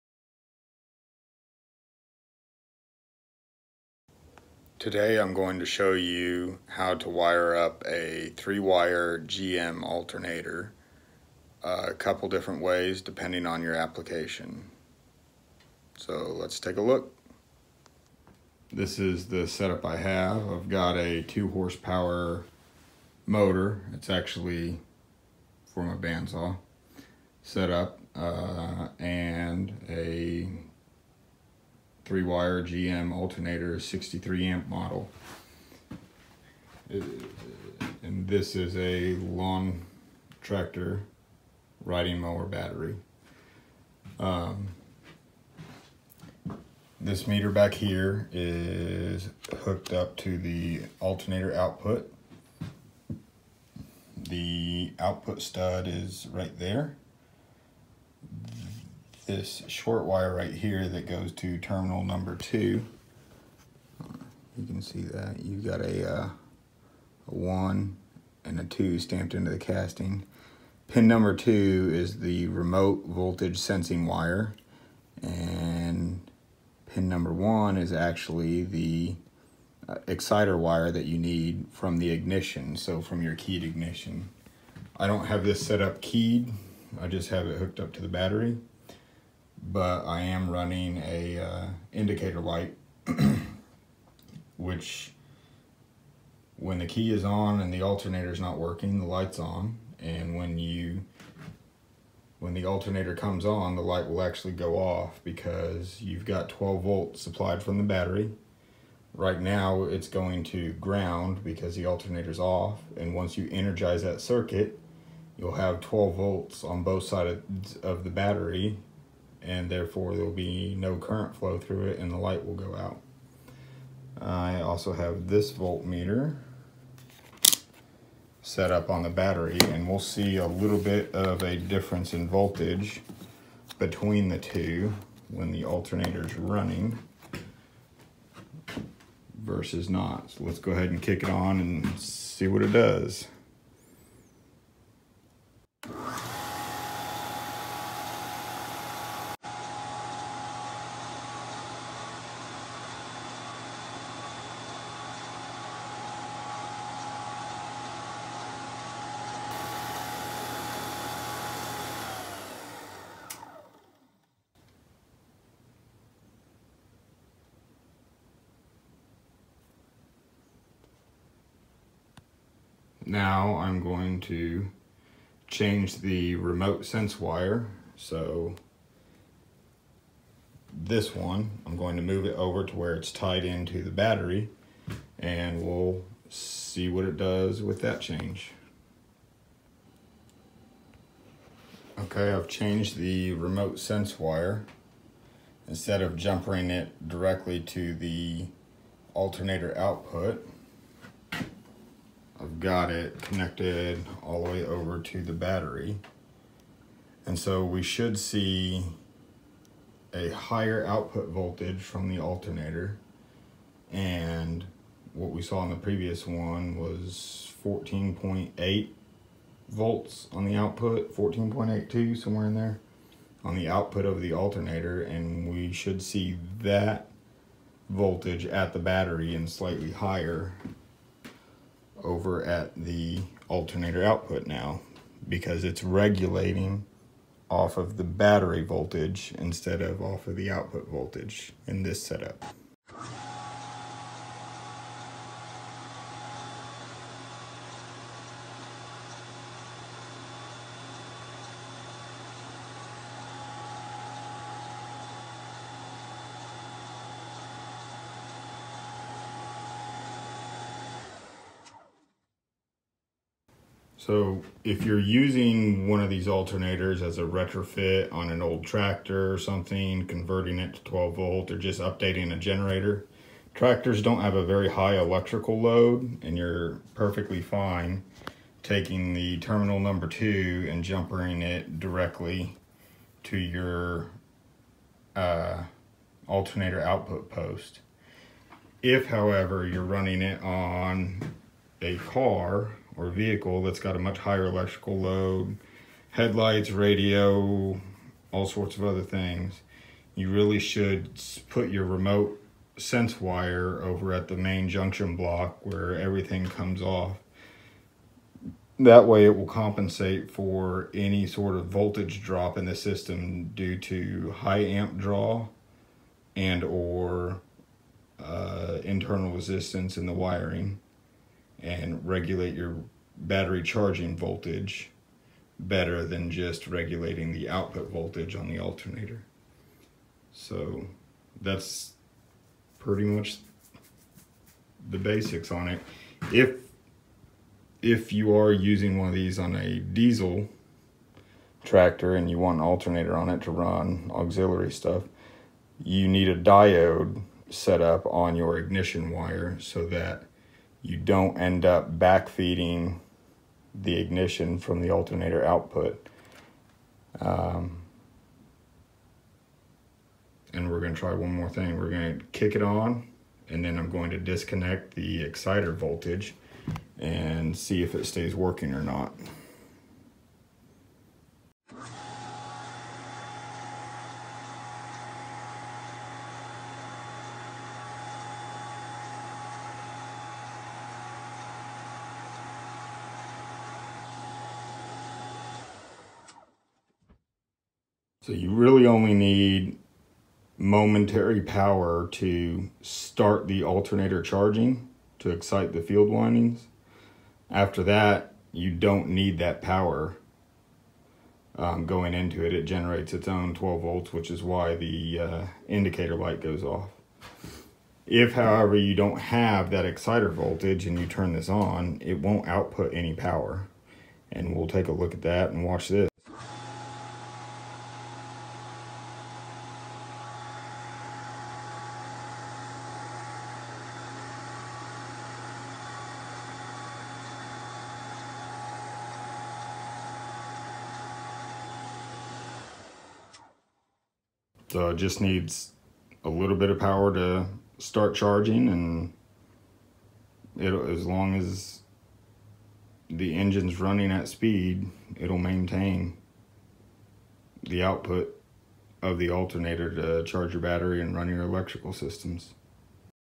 GM 3 wire alternator setup
How to wire a GM 3 wire alternator for a tractor vs a car, plus this setup doubles as a 63 amp fast battery charger.